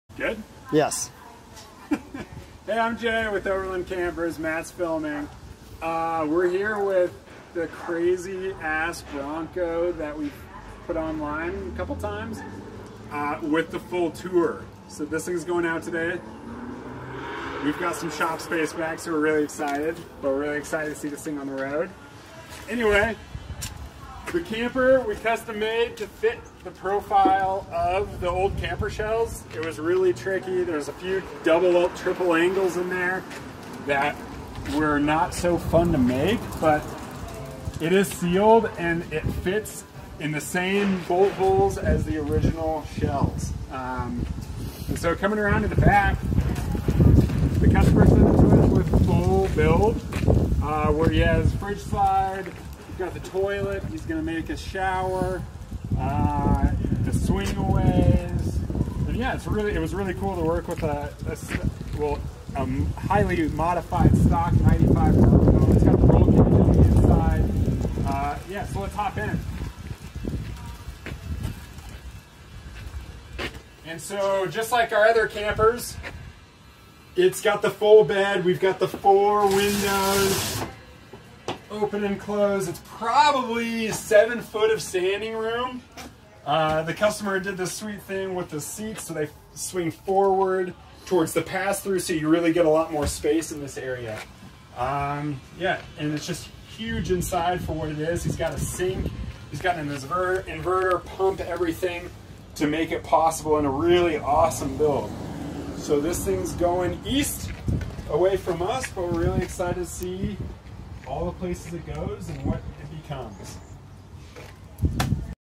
Custom '95 Bronco Camper
Quick tour of OVRLND Campers custom built Bronco camper.  Mounted straight to the stock shells mounting points created one sharp looking camper!